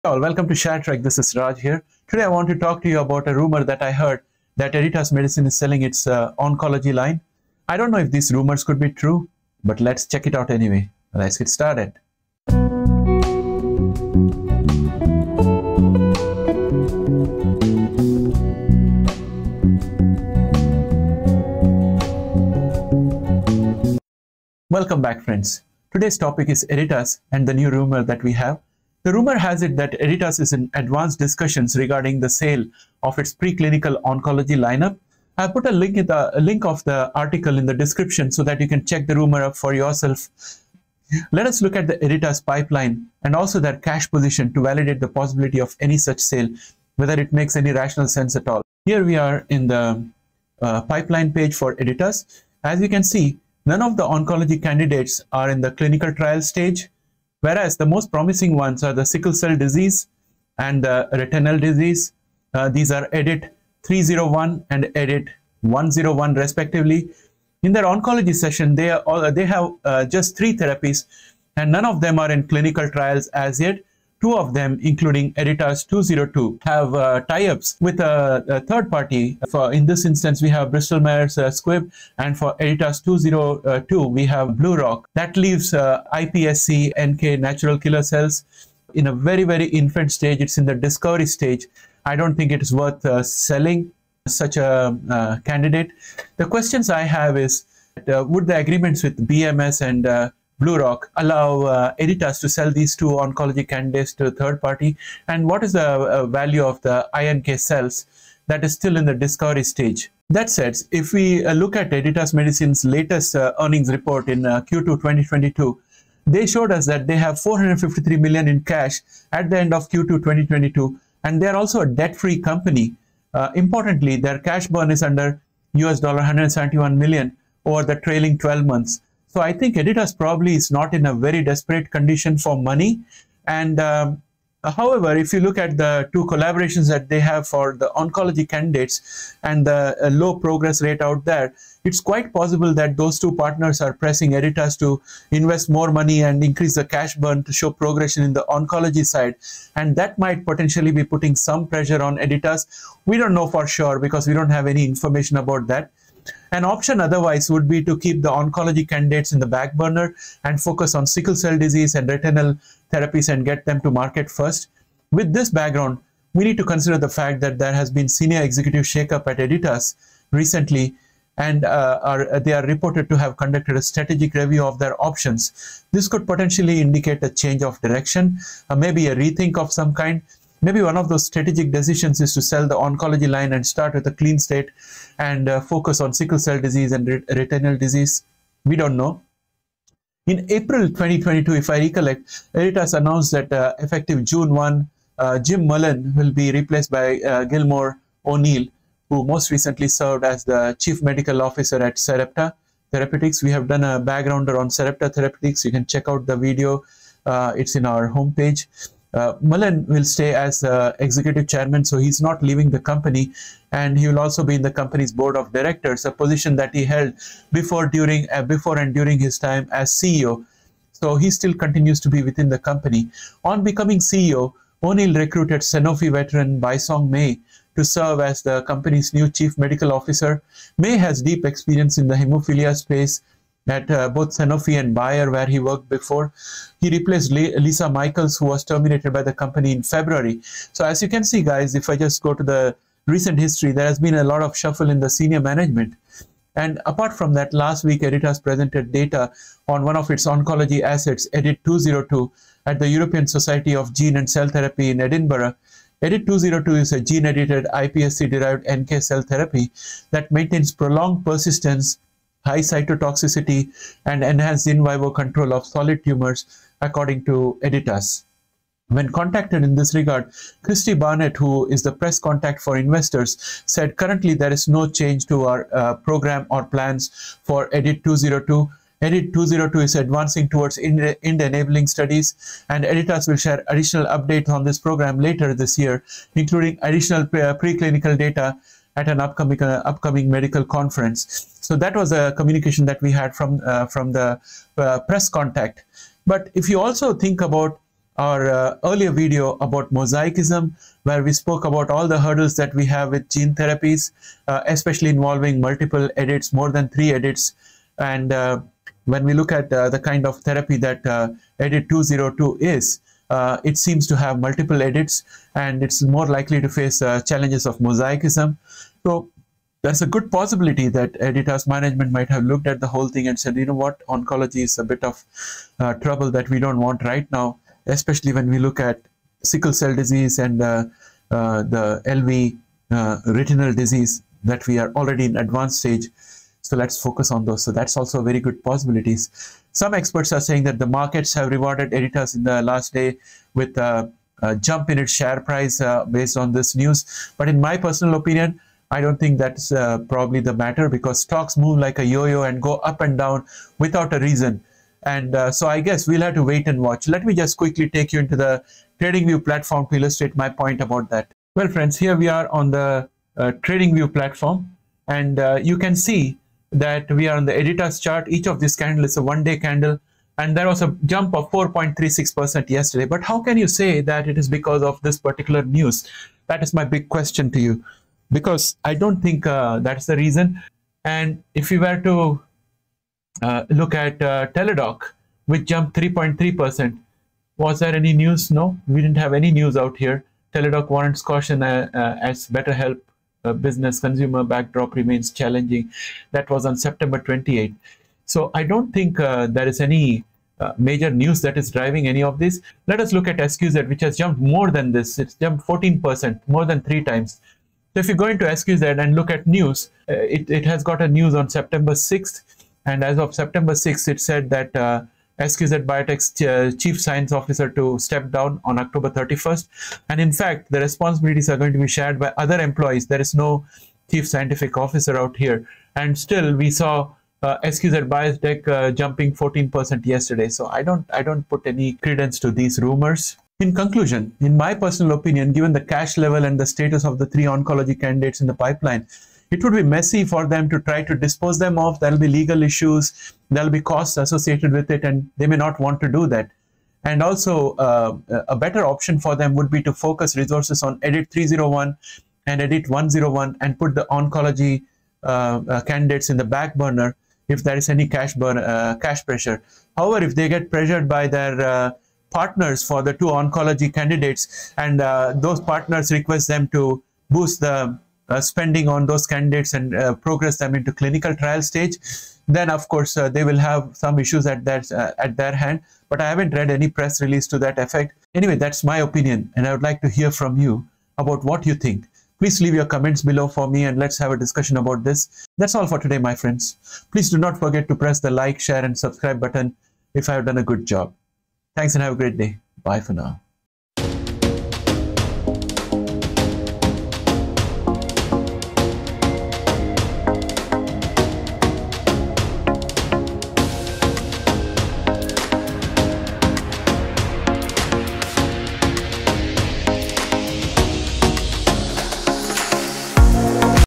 Rumor has it that Editas is selling oncology assets. Lets check this out!
In this video, I try to find if rumors that Editas is selling its oncology lineup is true and present my conclusions.

00:00:00 Introduction
00:12:10 Title track
00:24:01 Discussing the rumor and validating against pipeline & finances.
06:00:28 Debnking rumor around stock price responding positively to this rumor.
07:12:25 Conclusions